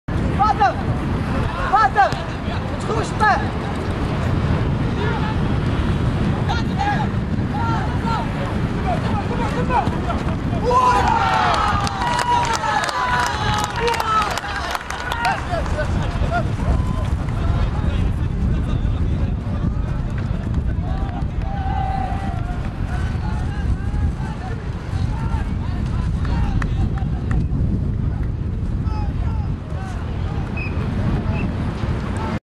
• Hamza En-naghimi Incredible goal ~ Season 2013-2014 | HD
Season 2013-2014 Match AMF vs Rachad Bernoussi.
This is the original version of the video.